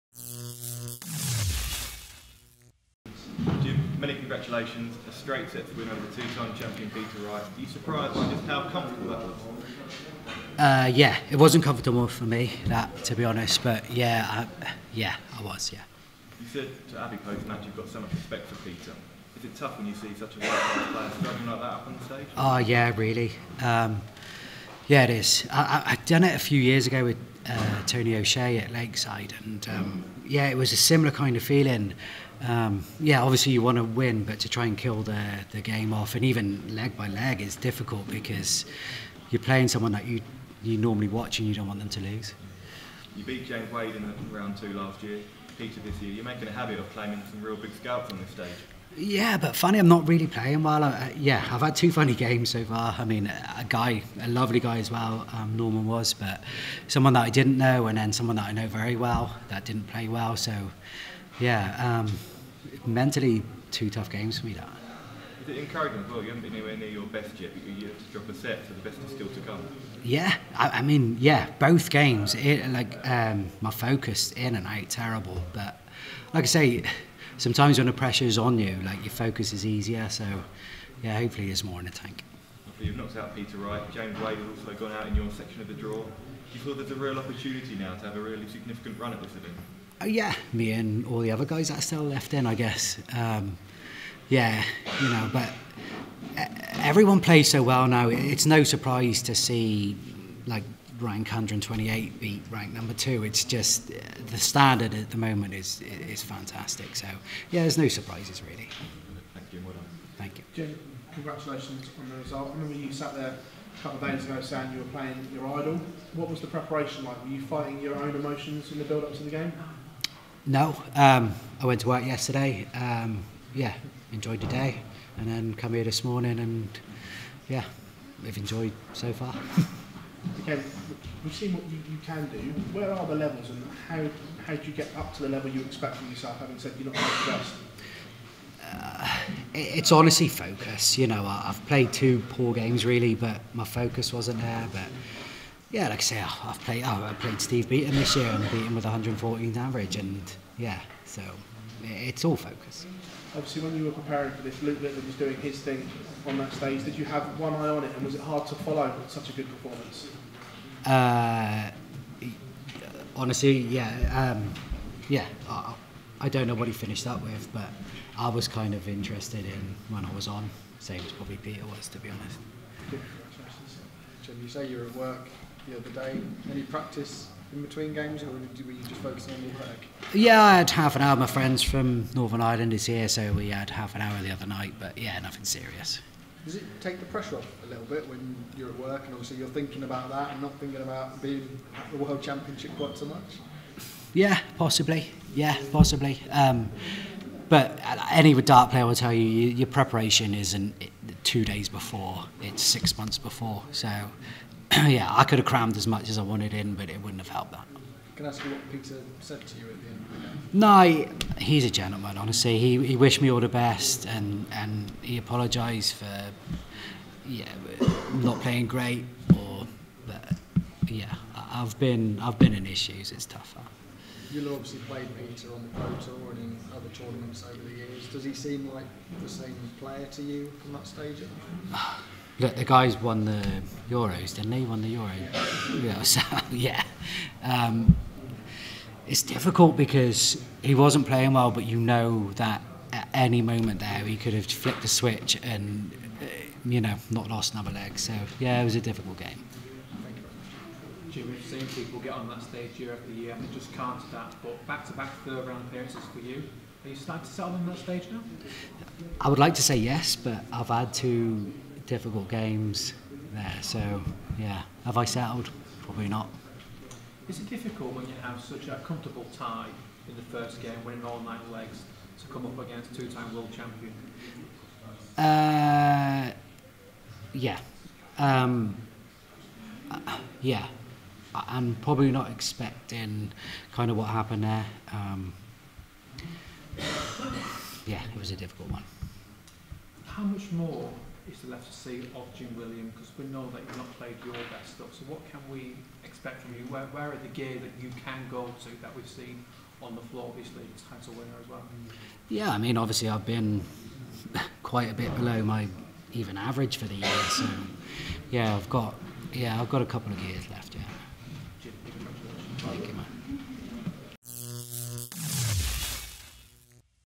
Jim Williams REVEALS Peter Wright's post-match words after crushing 3-0 victory
Welsh star Jim Williams gives his honest reaction to a 3-0 scalp of an out of sorts two-time World Champion Peter Wright to reach the Third Round of the World Championship.